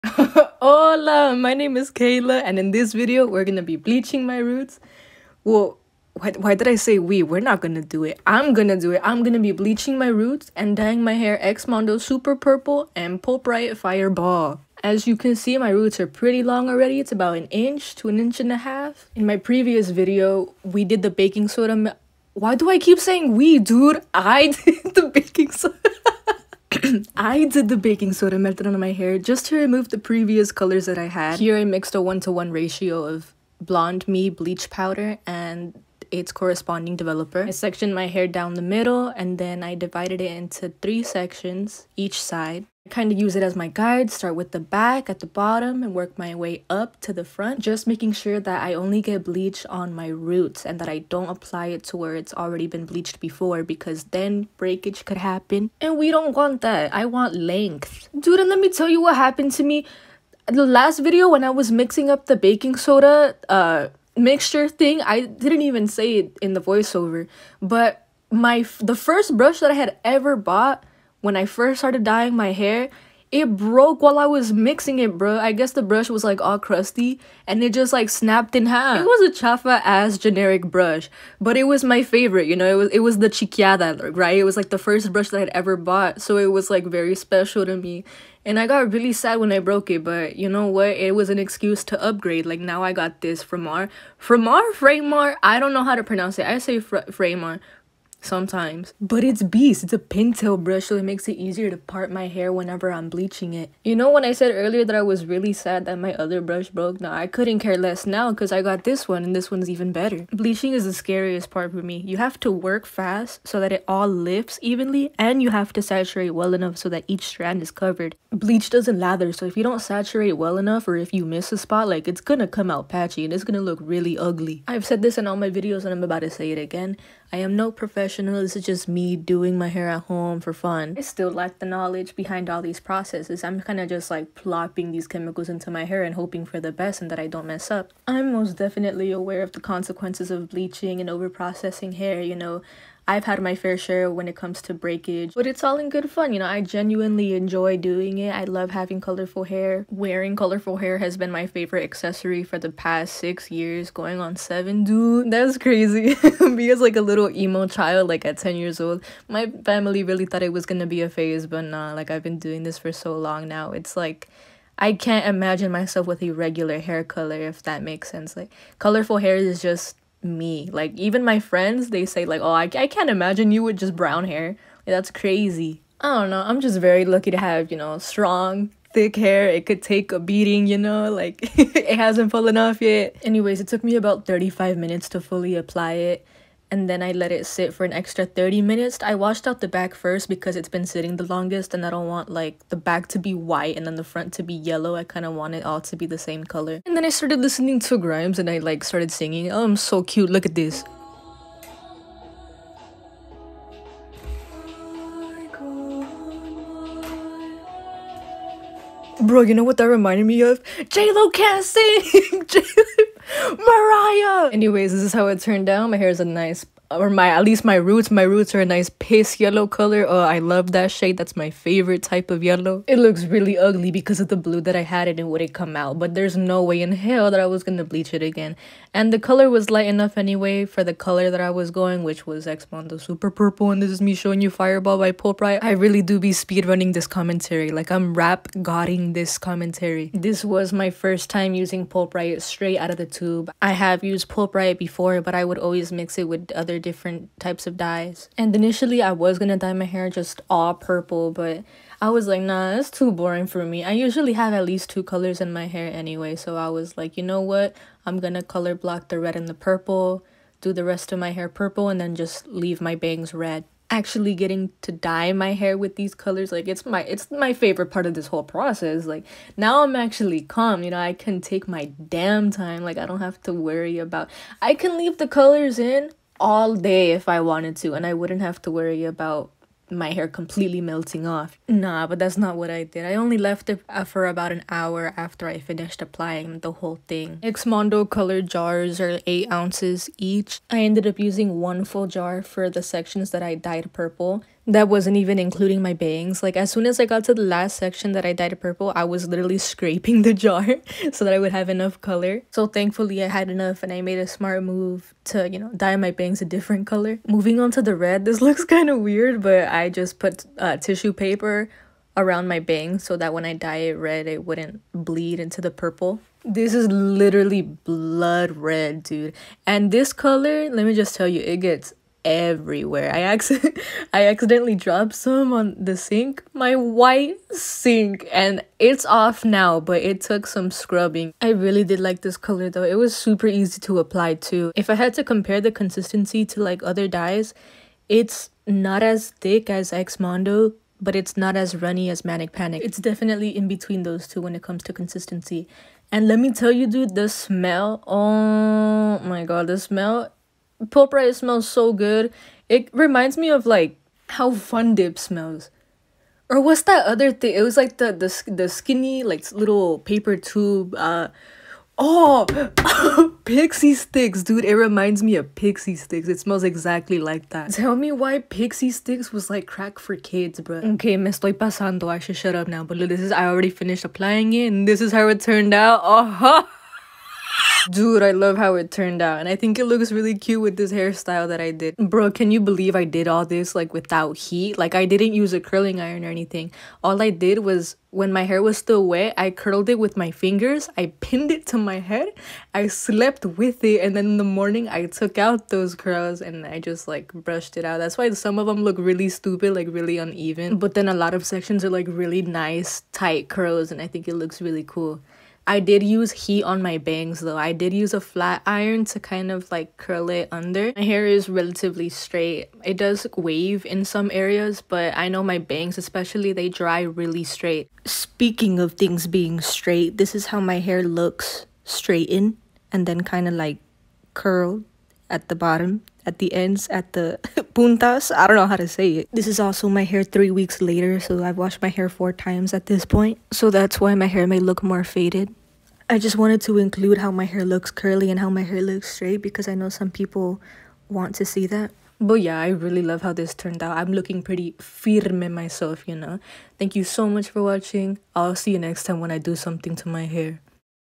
Dyeing my Hair Purple & Red || XMONDO Color Super Purple & Pulpriot Fireball
Hey, my name is Kaila, and In this video I bleach my roots using Schwarzkopf BlondMe and dye my hair using Xmondo’s Super Purple and Pulpriot’s Fireball.
I have used Super Purple before in my previous videos, but this is the first time I’ve tried Pulpriot straight from the tube.
*I am no hair expert, this video is not a “how to”, I just wanted to show what I do to my hair for fun.

Age: 16
Hair Instagram: @ammonicmedusa